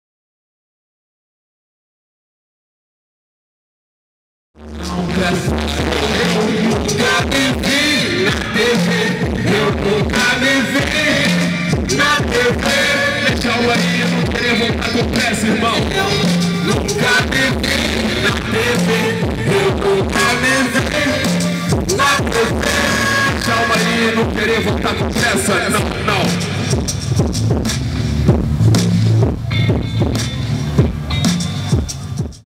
Back2Black 2013 - Thiago Elniño (Palco Estrombo)
Thiago Elniño, um dos vencedores do Concurso "Seja Artista do Palco Estrombo", se apresentando no Back2Black!